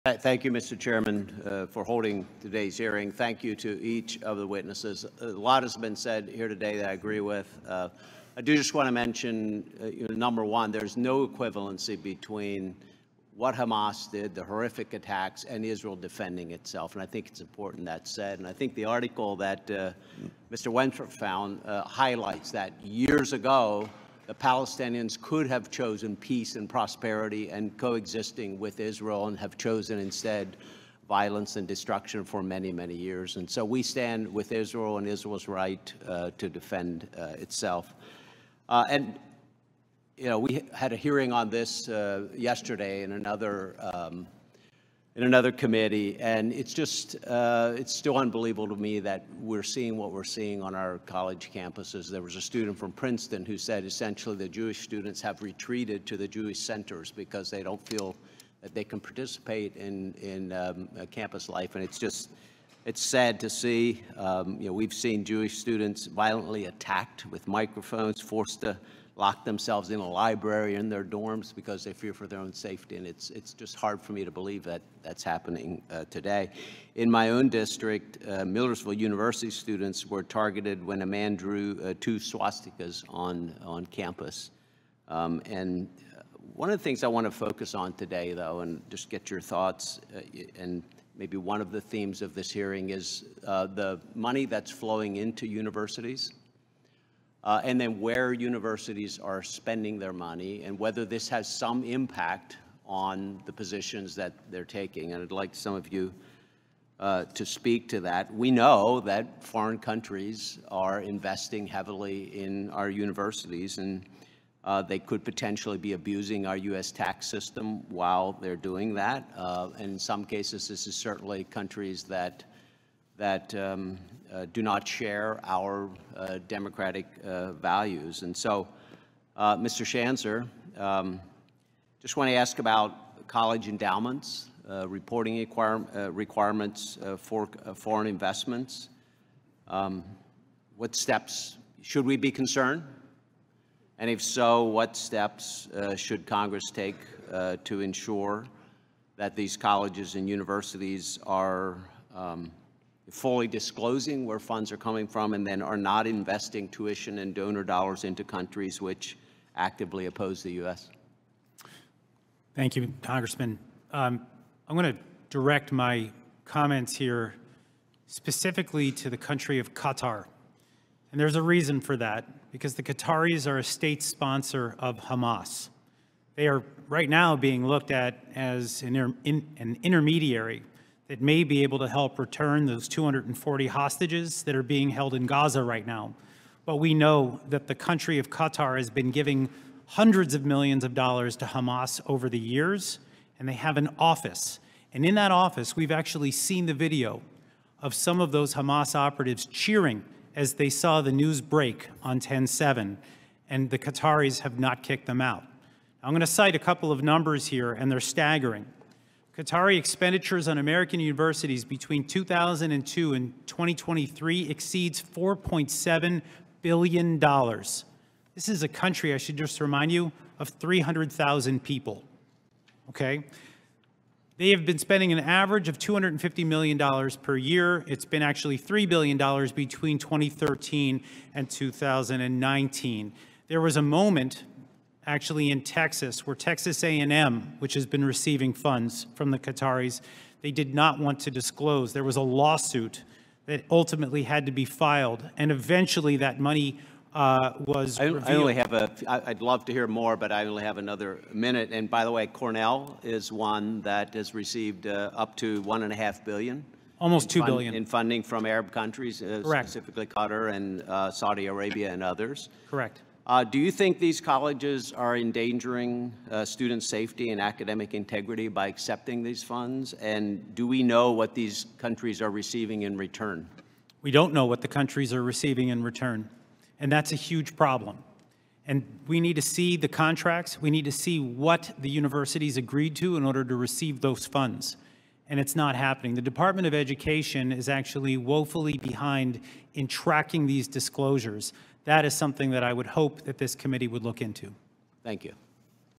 Rep. Smucker: Questions from Hearing on Antisemitism, Tax-Exempt Universities, and Terror Financing
Rep. Lloyd Smucker (PA-11) asks questions during the Wednesday, November 15, 2023 hearing of the Ways & Means Committee titled: "From Ivory Towers to Dark Corners: Investigating the Nexus Between Antisemitism, Tax-Exempt Universities, and Terror Financing.”